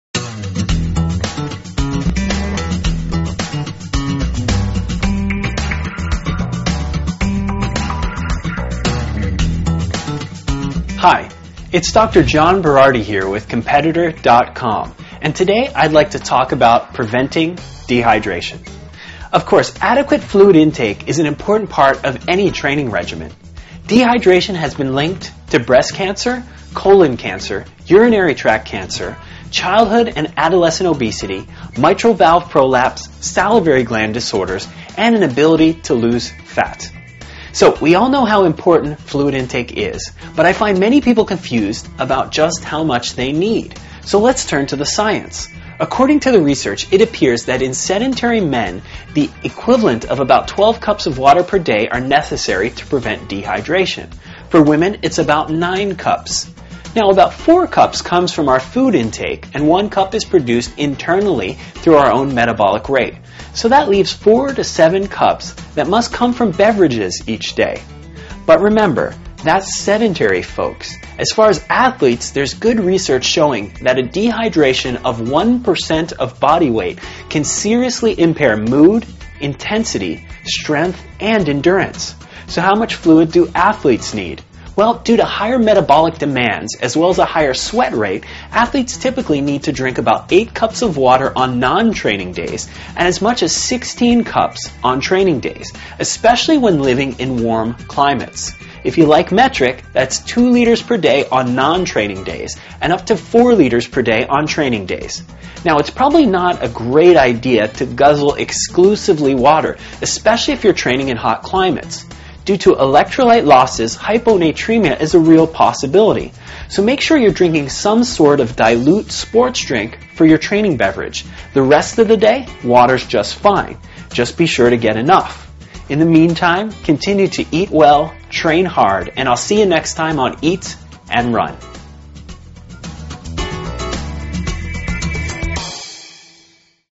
Eat And Run: Preventing Dehydration
Dr. John Berardi tells how dehydration can not only lead to reduced performance but it can also lead to various medical conditions. Learn a little more about the dangers of dehydration and how best to prevent it.